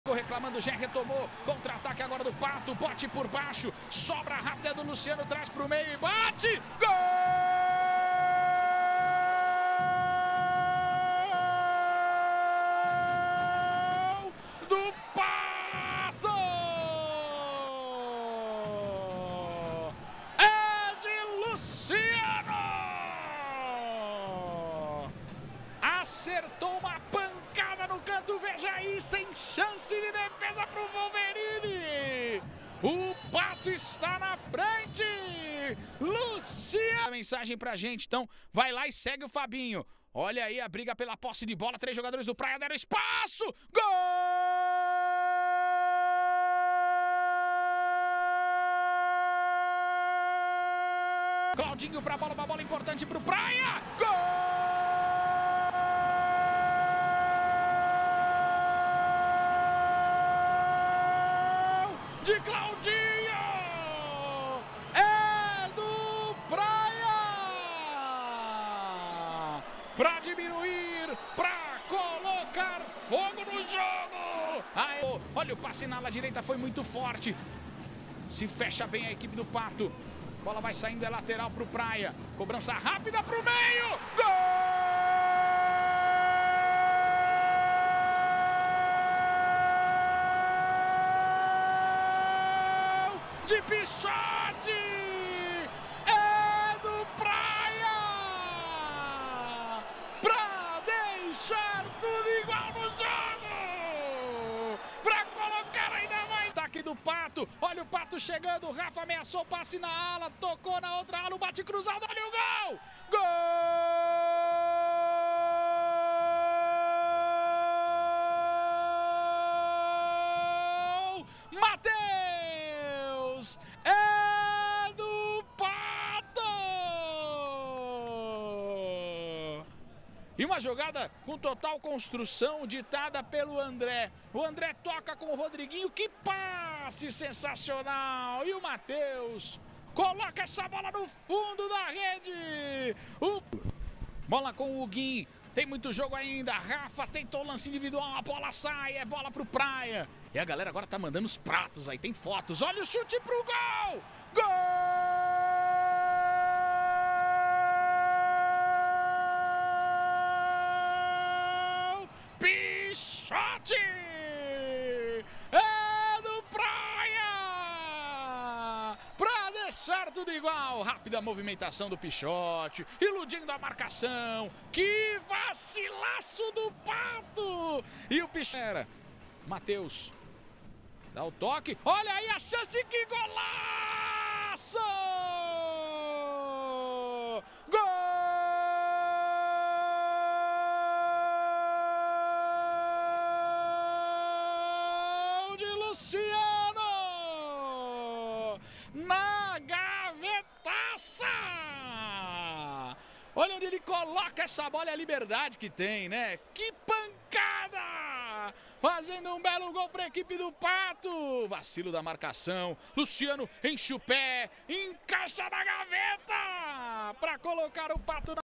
LNF 2022 - OITAVAS DE FINAL - VOLTA - GOLS - PRA 3 X 4 PAT
Gols:
Praia Clube - Claudinho e Pixote (2)
Pato - Rangel, Matheus e Luciano (2)
Transmissão @Bandsportsoficial e LNFTV